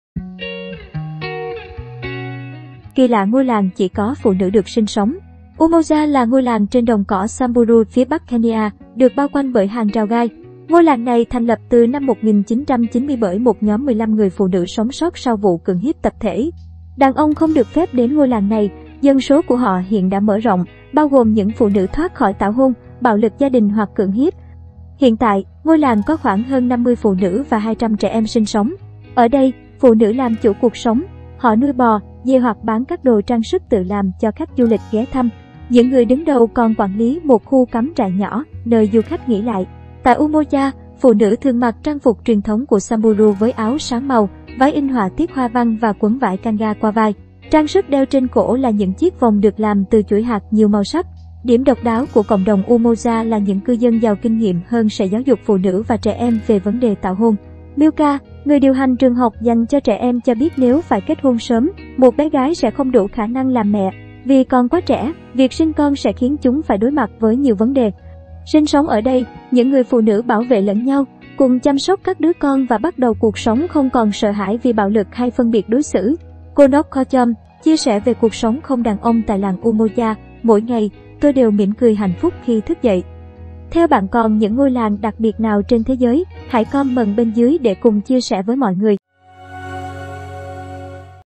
Kỳ lạ ngôi làng chỉ có phụ nữ được sinh sống
Umoja là ngôi làng trên đồng cỏ Samburu, phía bắc Kenya, được bao quanh bởi hàng rào gai. Ngôi làng này thành lập từ năm 1990 bởi một nhóm 15 người phụ nữ sống sót sau vụ cưỡng hiếp tập thể.